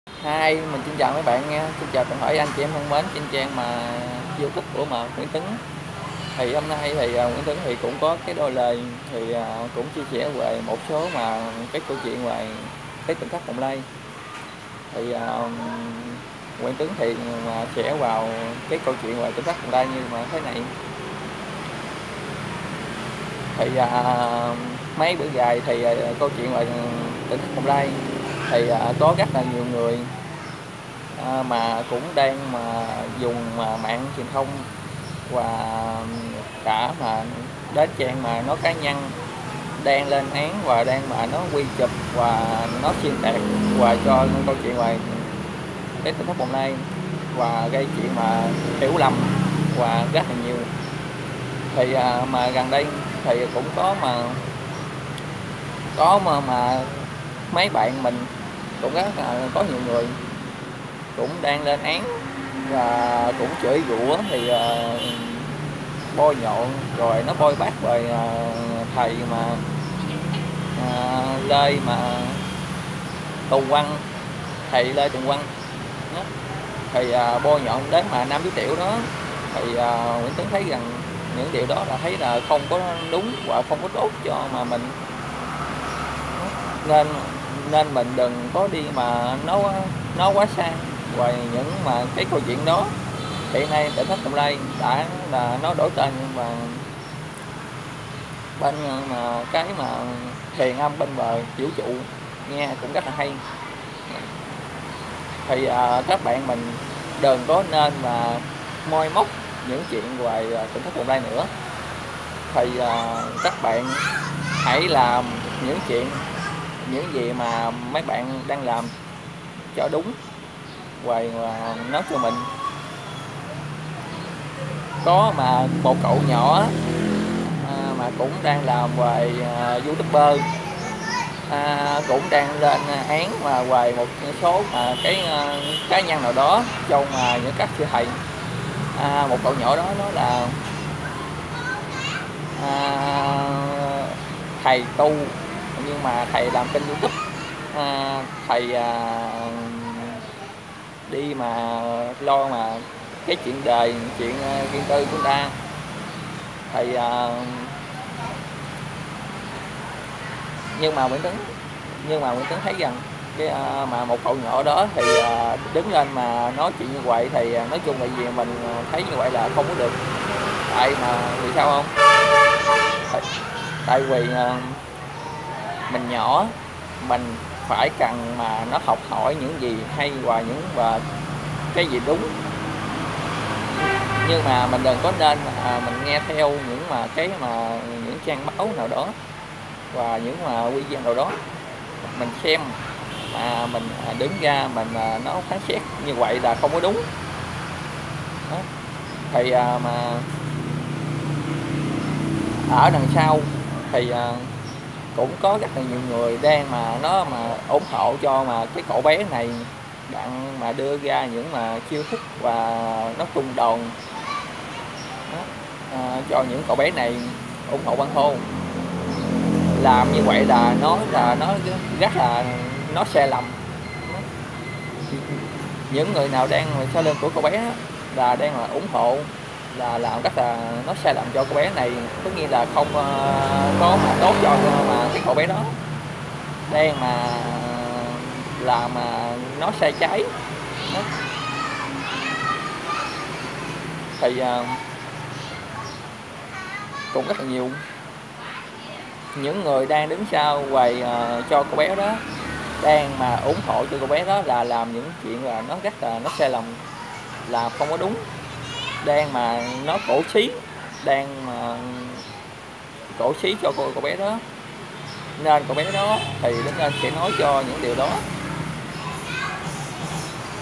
Đuôi lời gửi các bạn đừng nên xuyên tạc về tịnh thất bồng lai
Gần đây có một số người không hiểu về chuyện ở tịnh thất bồng lai nghe theo thông tin sai lệch sự thật về các chú tiểu ở trong chùa dại bảo rất tốt rất hây cho các em nhỏ